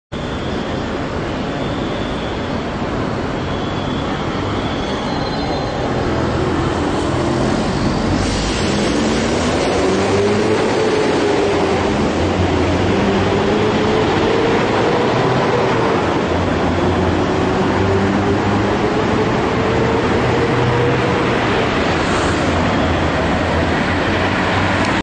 Copa do Mundo 2010 Avenida Paulista Praticamente Vazia
Copa do Mundo 2010 África do Sul   Avenida Paulista uma das Avenidas mais movimentada do Brasil estava praticamente vazio durante o Jogo da Seleção Brasileira